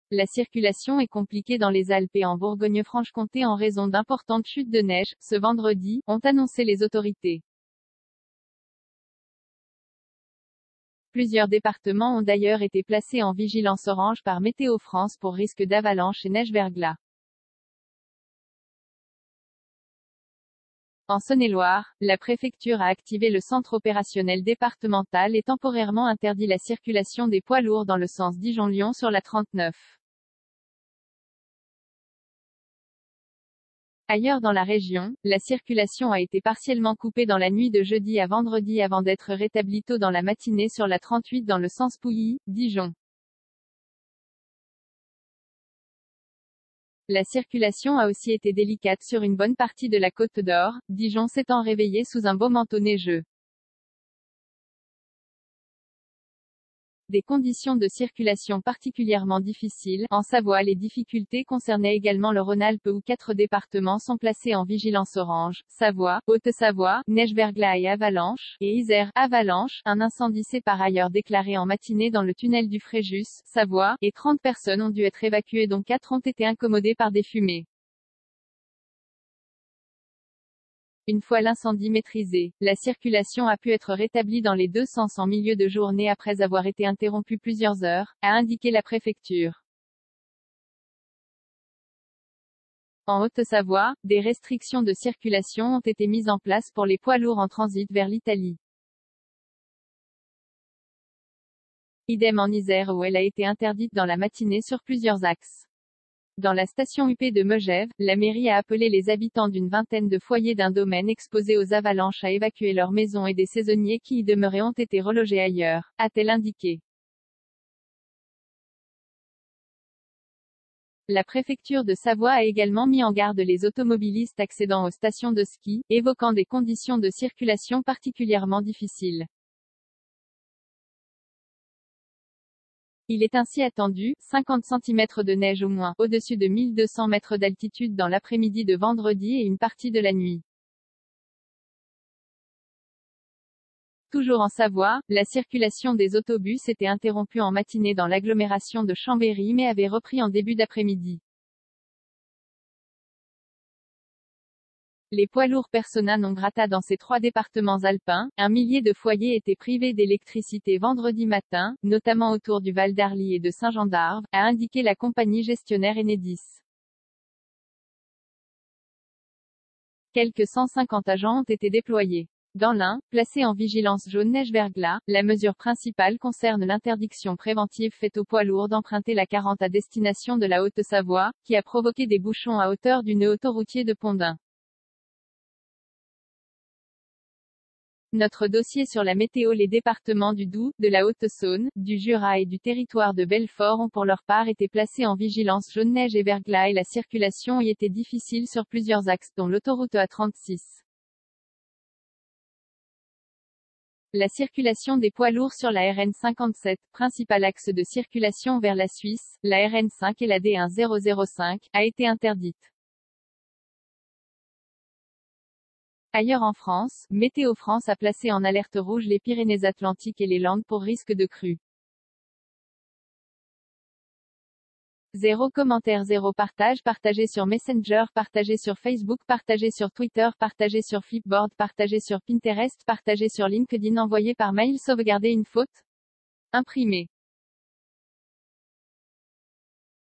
Météo : Des difficultés de circulation dans les Alpes et en Bourgogne-Franche-Comté en raison de la
La circulation est compliquée dans les Alpes et   en Bourgogne-Franche-Comté en raison d’importantes chutes   de neige, ce vendredi, ont annoncé les autorités. Plusieurs départements ont d'ailleurs été placés en vigilance orange par Météo France    pour risque d’avalanches et neige-verglas.    En Saône-et-Loire, la préfecture a activé le Centre opérationnel départemental et temporairement interdit la circulation des poids lourds dans le sens Dijon-Lyon sur l'A39. Ailleurs dans la région, la circulation a été partiellement coupée dans la nuit de jeudi à vendredi avant d’être rétablie tôt dans la matinée sur l'A38 dans le sens Pouilly/Dijon. La circulation a aussi été délicate sur une bonne partie de la Côte-d’Or, Dijon s’étant réveillée sous un beau manteau neigeux.  Des « conditions de circulation particulièrement difficiles » en Savoie   Les difficultés concernaient également le Rhône-Alpes où quatre départements sont placés en vigilance orange : Savoie, Haute-Savoie (neige-verglas et avalanches) et Isère (avalanches) Un incendie s’est par ailleurs déclaré en matinée dans le tunnel du Fréjus (Savoie) et 30 personnes ont dû être évacuées dont 4 ont été incommodées par des fumées. Une fois l’incendie maîtrisé, la circulation a pu être rétablie dans les deux sens en milieu de journée après avoir été interrompue plusieurs heures, a indiqué la préfecture. En Haute-Savoie, des restrictions de circulation ont été mises en place pour les poids lourds en transit vers l’Italie. Idem en Isère où elle a été interdite dans la matinée sur plusieurs axes.   Dans la station huppée de Megève, la mairie a appelé les habitants d’une vingtaine de foyers d’un domaine exposé aux avalanches à évacuer leurs maisons et des saisonniers qui y demeuraient ont été relogés ailleurs, a-t-elle indiqué. La préfecture de Savoie a également mis en garde les automobilistes accédant aux stations de ski, évoquant des « conditions de circulation particulièrement difficiles ». Il est ainsi attendu « 50 cm de neige au moins » au-dessus de 1.200 m d’altitude dans l’après-midi de vendredi et une partie de la nuit. Toujours en Savoie, la circulation des autobus était interrompue en matinée dans l’agglomération de Chambéry mais avait repris en début d’après-midi.  Les poids lourds persona non grata   Dans ces trois départements alpins, un millier de foyers étaient privés d’électricité vendredi matin, notamment autour du Val d’Arly et de Saint-Jean-d’Arves, a indiqué la compagnie gestionnaire Enedis. Quelque 150 agents ont été déployés. Dans l’Ain, placé en vigilance jaune neige-verglas, la mesure principale concerne l’interdiction préventive faite aux poids lourds d’emprunter l'A40 à destination de la Haute-Savoie, qui a provoqué des bouchons à hauteur du nœud autoroutier de Pont-d’Ain.   Notre dossier sur la météo   Les départements du Doubs, de la Haute-Saône, du Jura et du Territoire de Belfort ont pour leur part été placés en vigilance jaune neige et verglas et la circulation y était